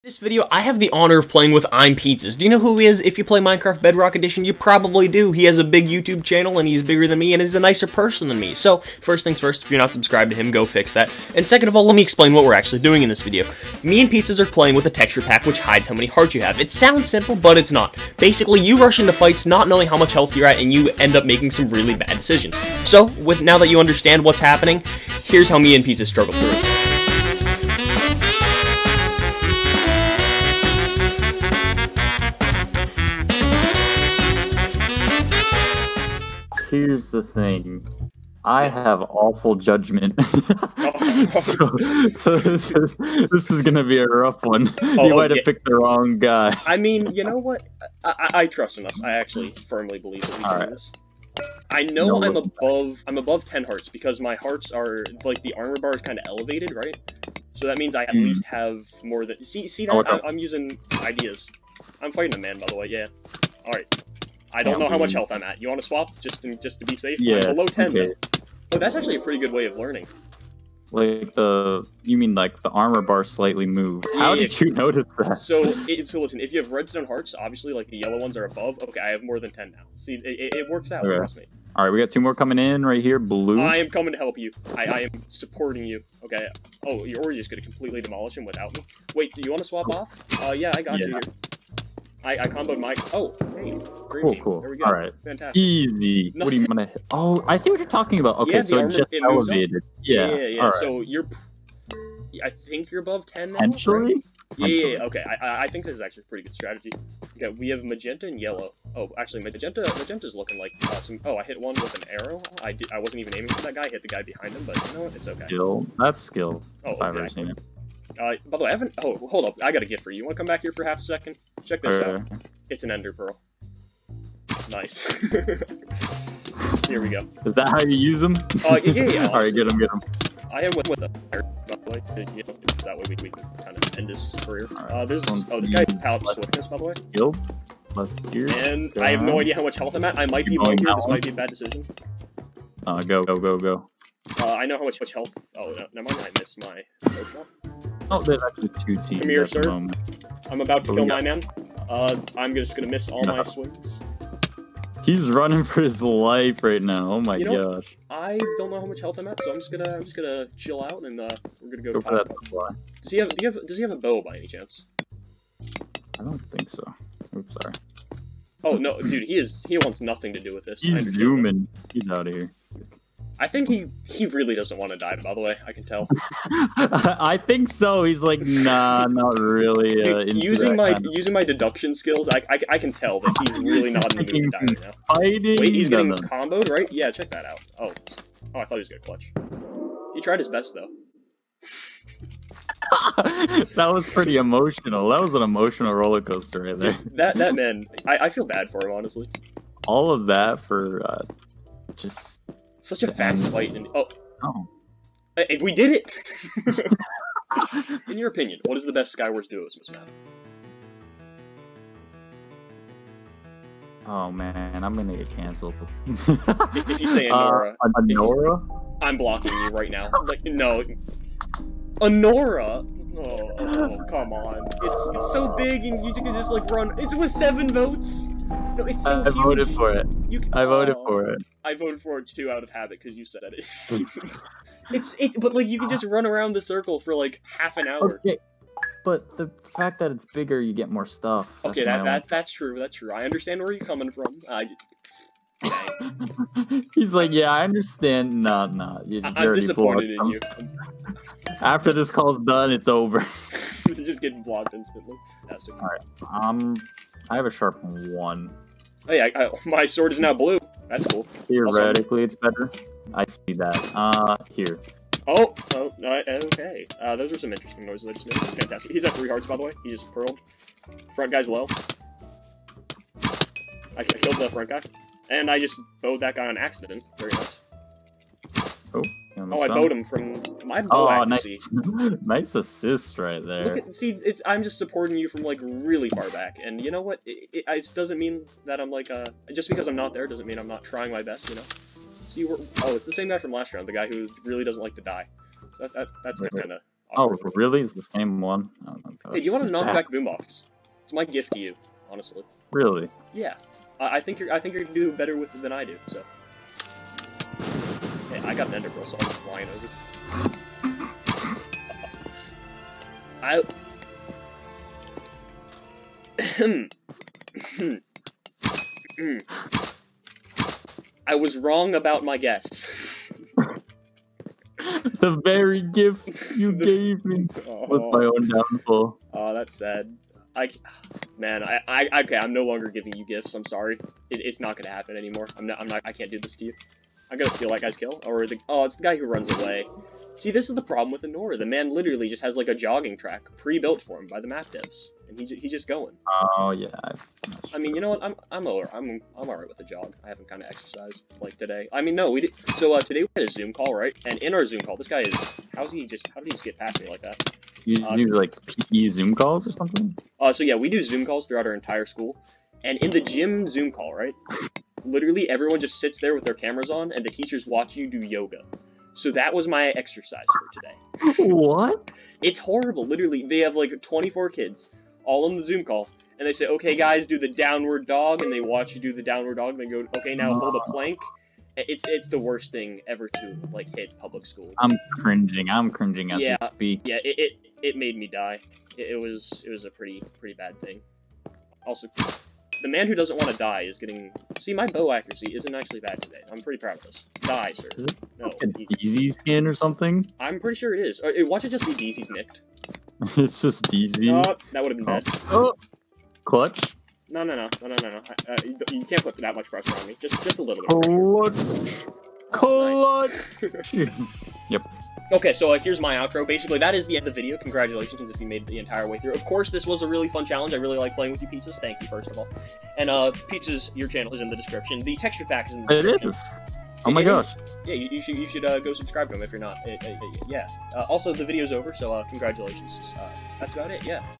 Can I win without seeing my Health? (ImPizzas)
SHOUTOUT TO PIZZAS FOR LETTING ME LEACH OFF HIS CLOUT

On an actual note though Thanks pizzas, I had a great time lol

Try the challenge out for yourself, tell me how it goes!